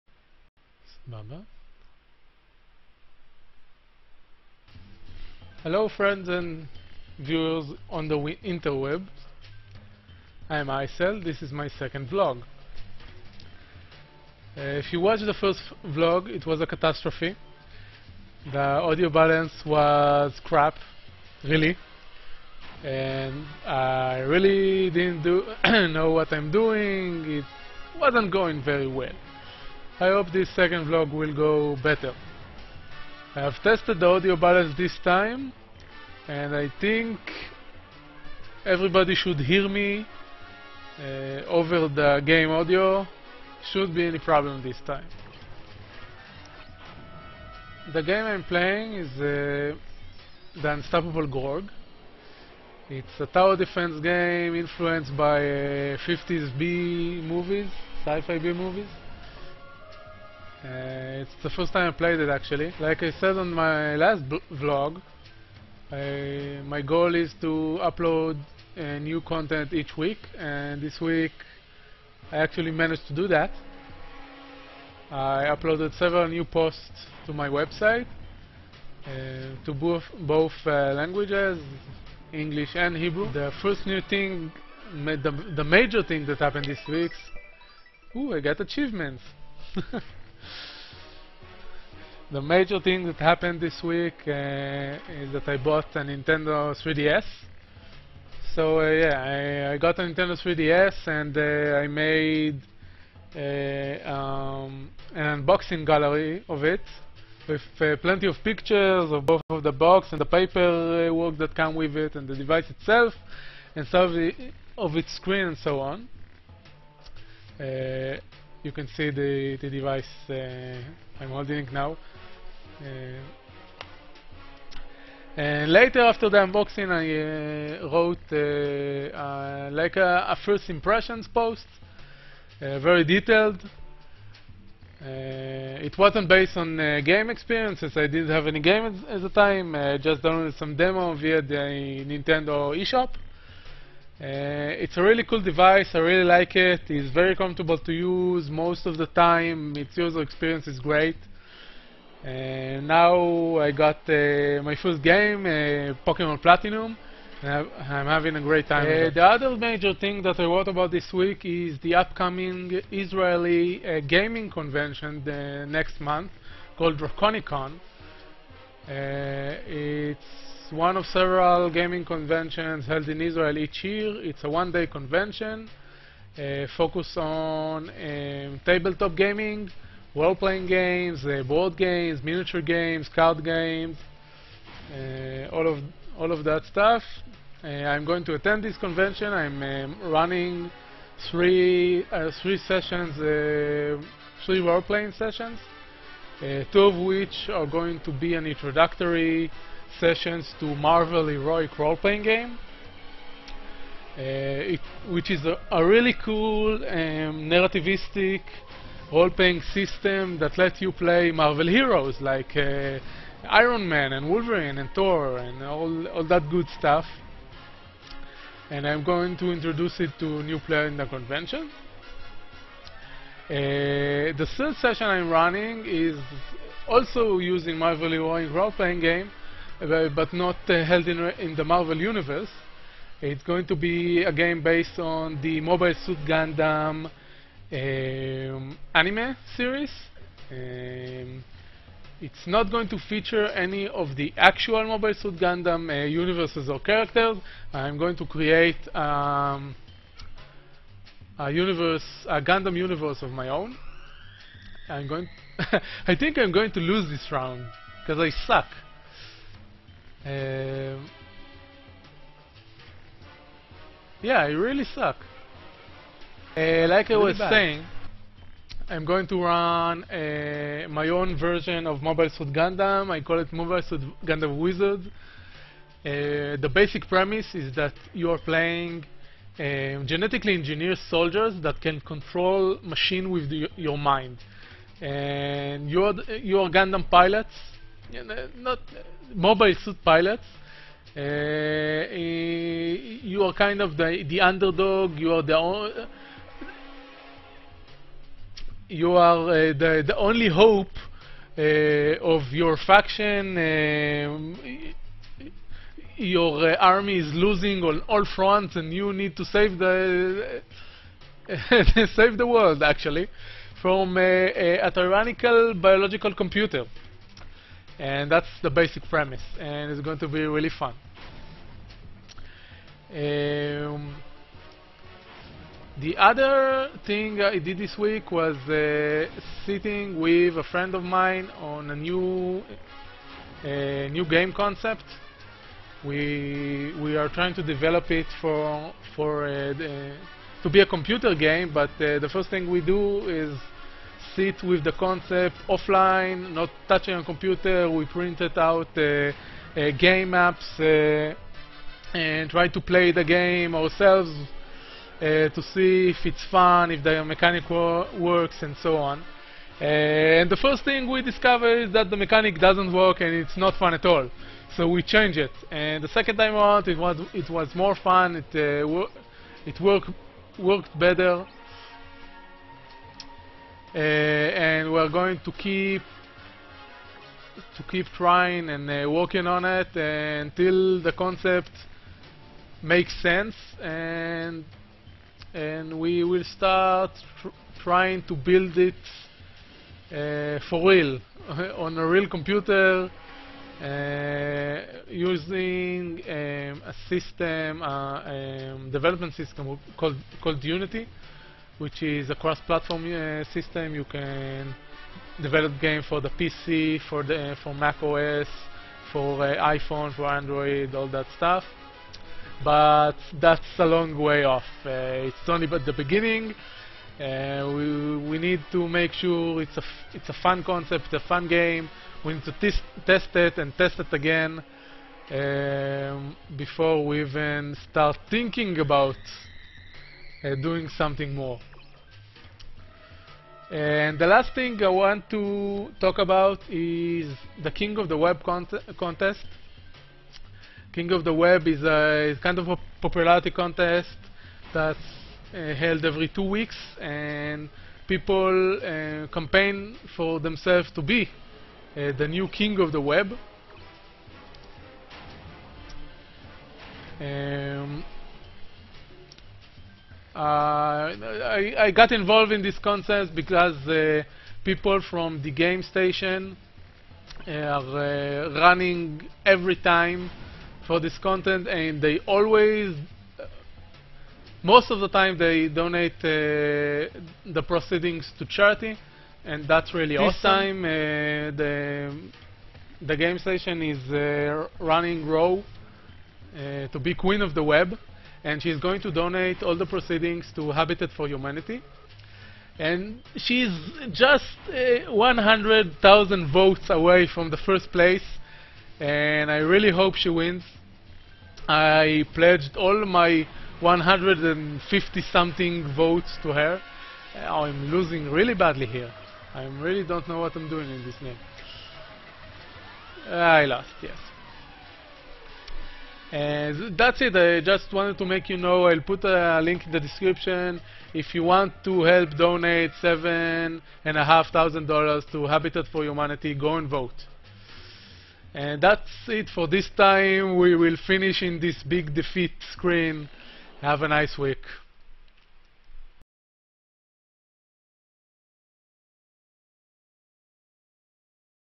Icel Vlogs [Week 2] - Website Updates, 3DS & Testing Your Game Concepts
This second vlog featured stuff I worked on the past week.

If you read Hebrew and wish to read more about the DroconiCon gaming event, the link below is for you.

Vote for Ro in King of the Web and help rise money for charity: